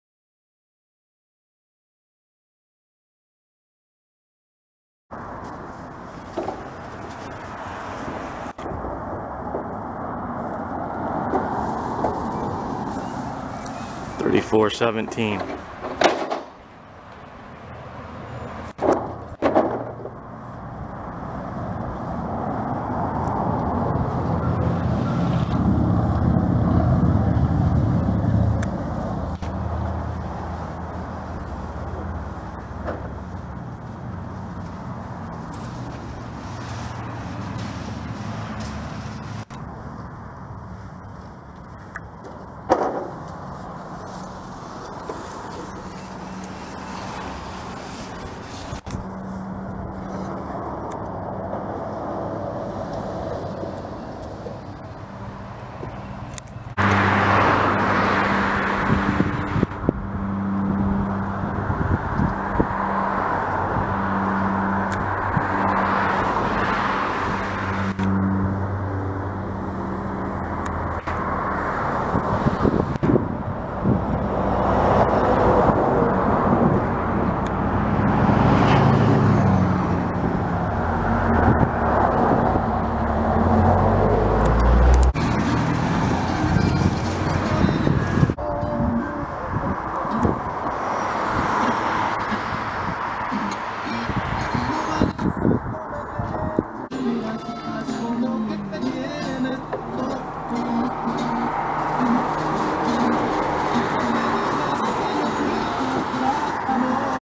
Florida still SNOWING in town of Zephyrhills - Cause: styrofoam - Tried but attempts fail to stop it
They tried but attempts fail to stop the SNOWING in town of Zephyrhills Florida
CAUSE: STYROFOAM from construction site.

City of Zephyrhills Code Enforcement:
    Joel Bacon
    Code Enforcement Officer

    Mike Sutton
    Code Enforcement Officer

    Wendy Heimes
    Fire Inspector

City of Zephyrhills City Manager:
William C. Poe, Jr.